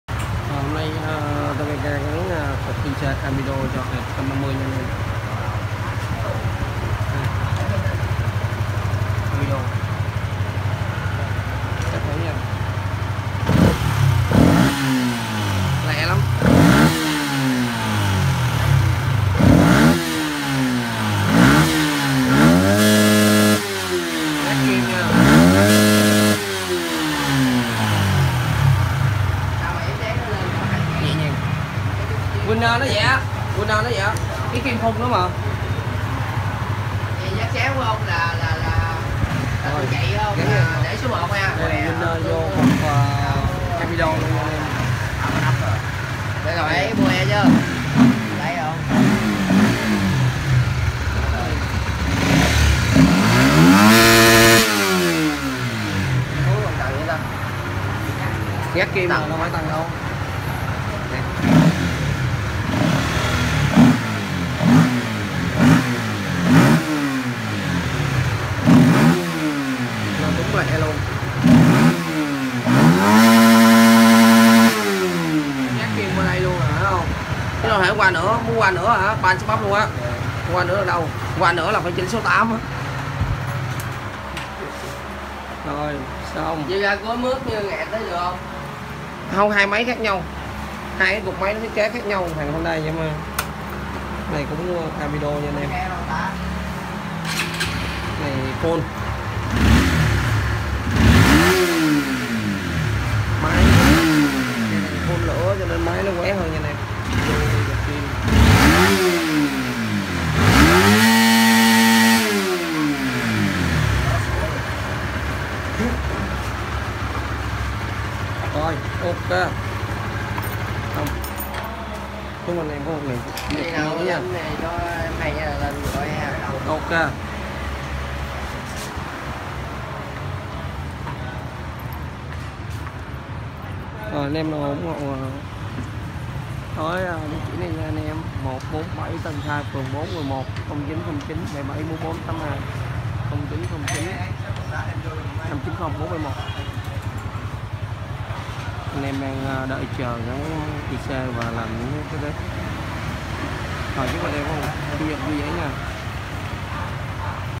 Exciter 150cc 2016 Vô ECU Mở Hết Tuor Máy Chạy Bốc Êm Ái Tại TBK Team 147 Tân Khai P4 Q11 Bảo 09097
Exciter 150cc 2016 Vô ECU Mở Hết Tuor Máy Chạy Bốc Êm Ái Tại TBK Team 147 Tân Khai P4 Q11 Bảo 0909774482